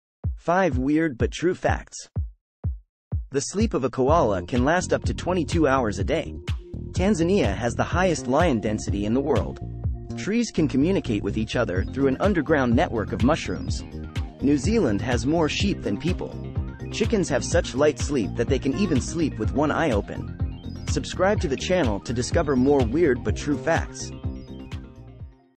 5 Extraordinary Nature Facts That Will Blow Your Mind!👍
Discover Fascinating Facts: Koala's Sleep, Tanzania's Lions, Tree Communication, New Zealand's Sheep, and Chicken Sleep Habits! 🐨🦁🌳🐑🐔 Dive into the intriguing world of wildlife and nature with this captivating YouTube short. Learn about the astonishing sleep patterns of koalas, the impressive lion population in Tanzania, the secret communication among trees through underground fungal networks, New Zealand's unique sheep-to-human ratio, and the curious ability of chickens to sleep with one eye open. Join us for an educational and entertaining exploration of these incredible natural phenomena. Don't miss out on this must-watch video! 🌍🎥

massimo13